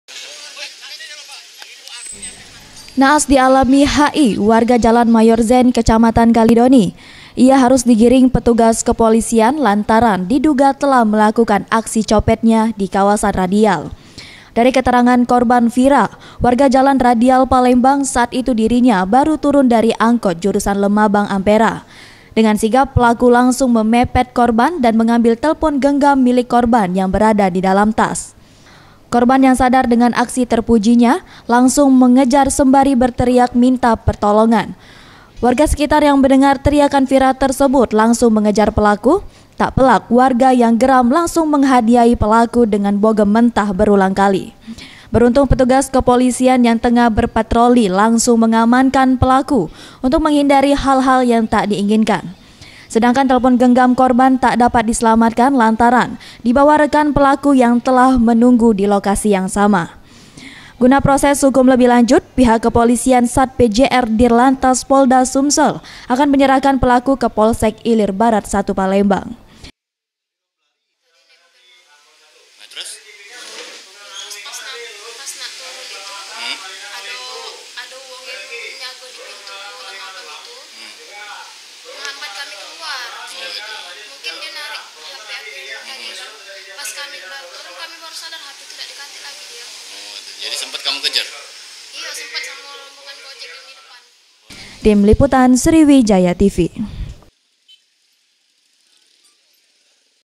1 Tersangka Pencopet Beroperasi Di Radial Dipukuli Warga
Tetap selalu waspada bila anda menaiki maupun turun dari kendaraan umum agar tak menjadi korban pencopetan seperti yang dialami Vira warga Jalan Radial. Ketika dirinya baru turun dari angkot jurusan Lemabang-Ampera, ia dipepet oleh seorang pria diduga ingin melakukan pencopetan.